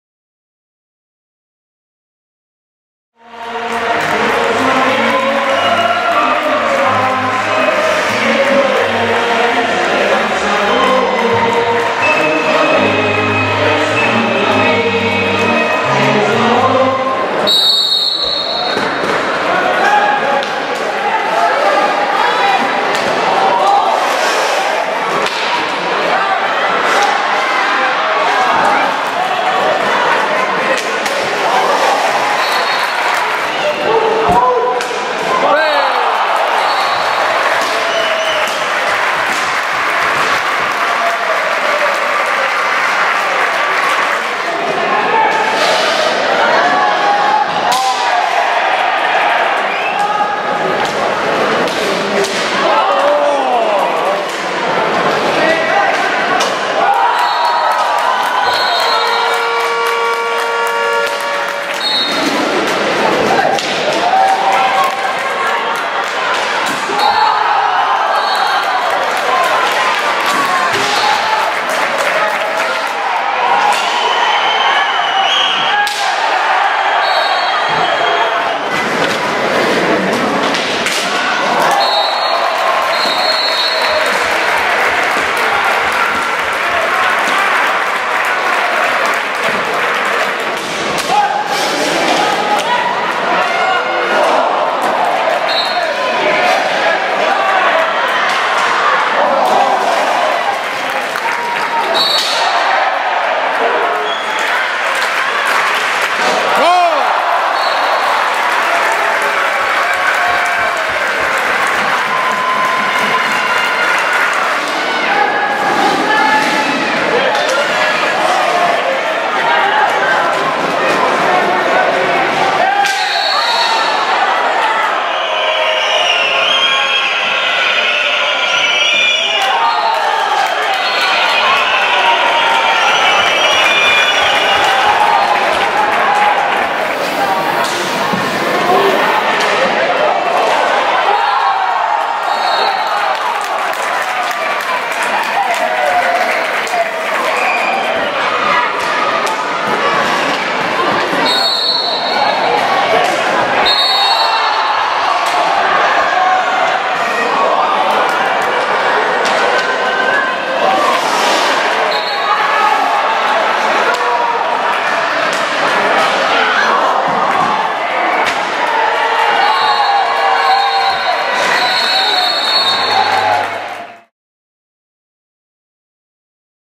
160521 - Resum @clubpativic 3-4 @okcevendrell
Resum del partit de la 28a jornada de l'OK lliga entre l'equip local del Club Patí Vic i el Moritz CE Vendrell amb el resultat final de 3 gols a 4 pel Vendrell.